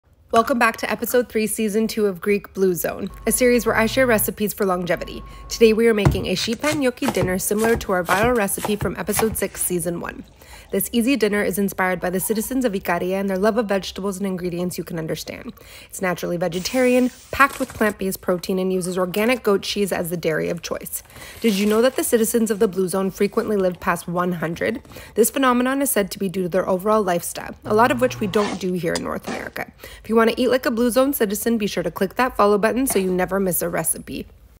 Simple Baked Gnocchi Sheet Pan Recipe
Welcome Back to Episode 3/10, Season 2 Of Greek Blue Zone.  A series where I’ll be sharing 👩🏻‍🍳🔪recipes for longevity. Today we are making a Simple Sheet Pan Gnocchi Recipe.

Make this dish if you’re ever in a pinch — it takes minutes to pull together and will satisfy the whole family.  Inspired by the flavours of Greece and incorporated the rustic pillowy flavor sponges from Italy! Craving Shrimp, add them, want Chicken, add them! Want more beans, add them (butter beans would be insane)! You get my point, this dish is versatile and FORGIVING!

Here is everything you need to become a Blue Zone Sheet Pan Gnocchi Dinner Expert 👩🏻‍🍳

Ingredients —

19 oz can chickpeas, drained and rinsed
2 small zucchini, chopped
1 fennel bulb, sliced thin
14 oz can artichoke hearts, drained and rinsed, then chopped
500 g store-bought potato gnocchi
4 shallots, sliced thin

Greek-inspired citrus dressing:

1/2 cup olive oil
1/2 tbsp dried oregano
1/2 tsp salt
1/4 tsp pepper
3 tbsp orange and/or blood orange juice (from about 1 large or 2 small oranges
1/2 tbsp red wine vinegar
2 small garlic cloves, pressed
100 g goat cheese, crumbled

Toppings:

100 g goat cheese, crumbled
3 cups arugula
1/2 cup finely chopped green olives
olive oil and salt

Instructions —

1. Preheat the oven to 400F and line a large baking sheet with parchment paper.
2. Mix the Greek dressing ingredients together and set aside.
3. To the sheet pan add: rinsed chickpeas, chopped zucchini, sliced fennel, chopped artichokes, gnocchi and sliced shallots.
4. Drizzle the dressing over the ingredients on the pan and toss to ensure everything is very well coated.
5. Bake for 25 minutes, tossing gently at the 15 minute mark. After 25 minutes crumble the goat cheese over top of all the ingredients on the pan and bake for 5 more minutes.
6. Top with arugula and green olives and a drizzle of olive oil and salt if desired. Serve immediately.